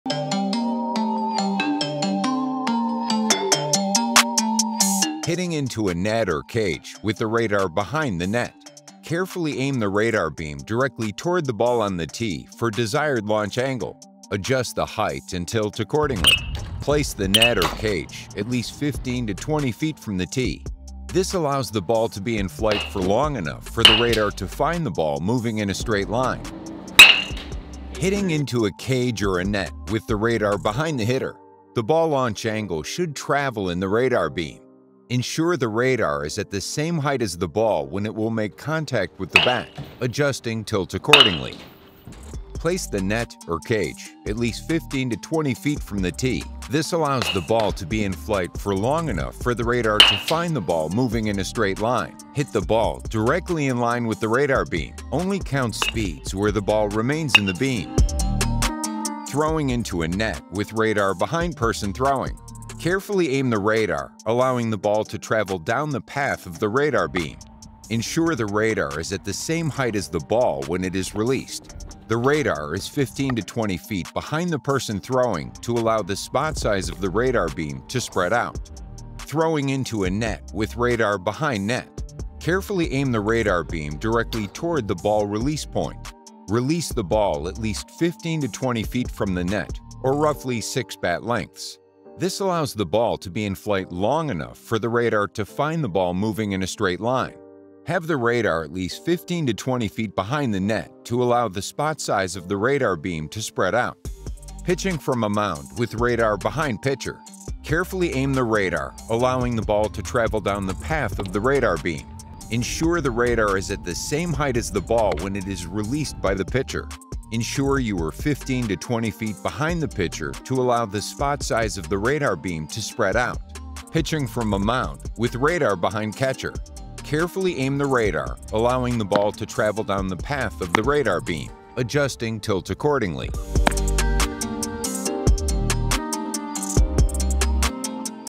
Pocket Radar Set Up Tutorial: Baseball
Learn the basics of how to properly set up your Pocket Radar device to capture baseball pitching, throwing and hitting ball exit velocities.